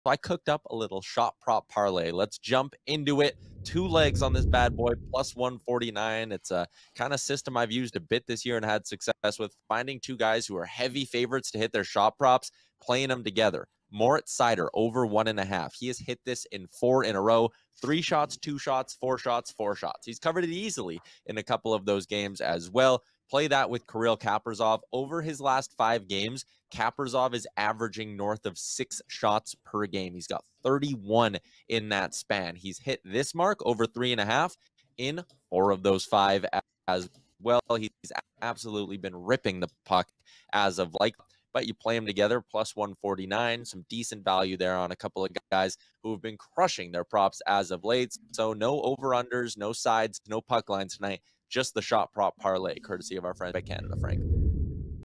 NHL DAILY BETS : March 8, 2023 | Daily Faceoff Live
As a result of the smaller NHL slate,
Yyler Yaremchuk put together a Shot Prop parlay for his PointsBet Canada Daily Bets!